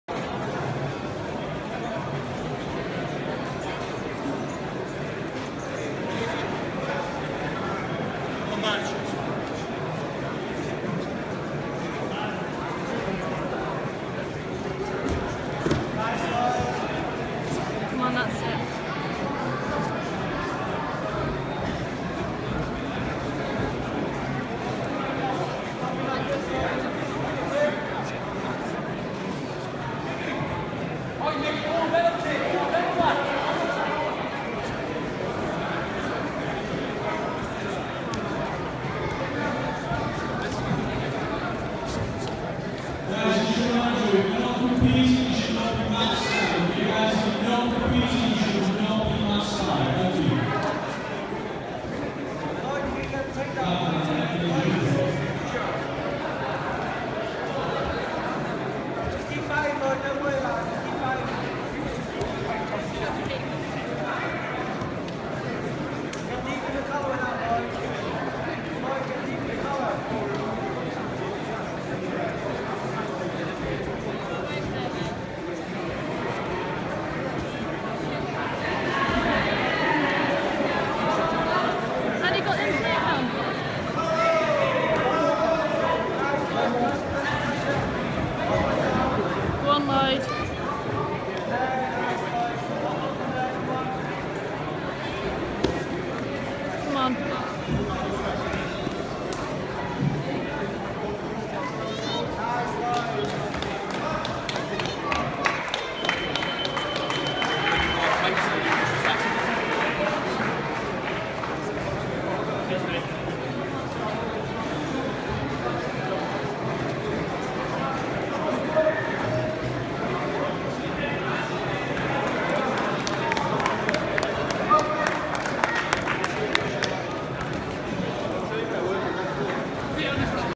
Lloyd Cooper Vs James Doyle - Scottish BJJ Open
Lloyd Cooper( Combat Base )  Vs James Doyle ( Infinity BJJ) - Brownbelt Middleweight final - Scottish BJJ Open